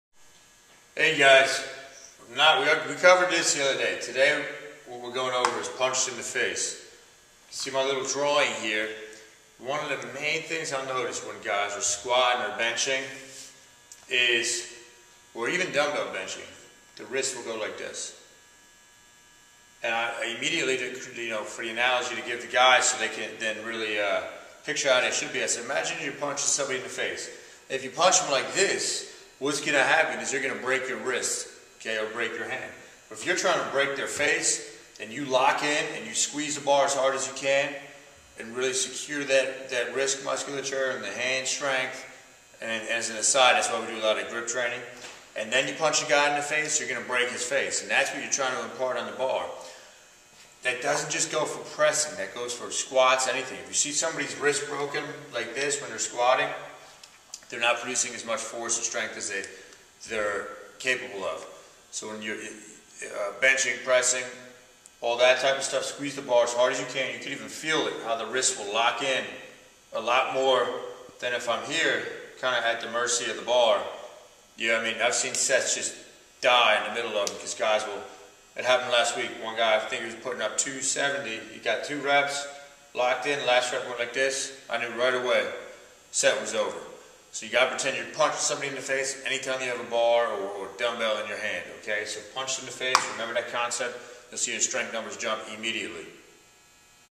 MEATHEAD SCIENCE
How to instantly increase your strength and power output by getting ready to punch someone in the face.